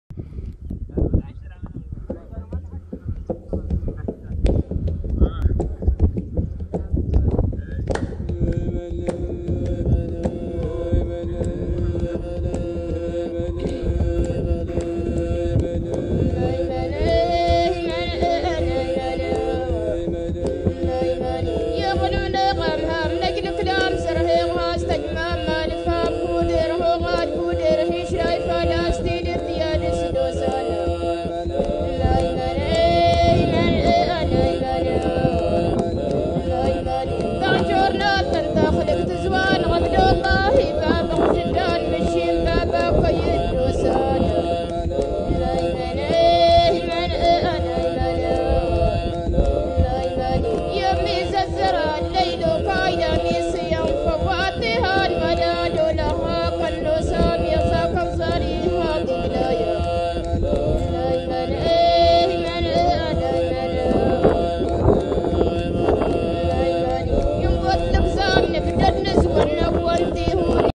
NIGER : le président Bazoum à Tesker
@54ETATS a suivi le Président de la République du Niger dans son village natal à Tesker.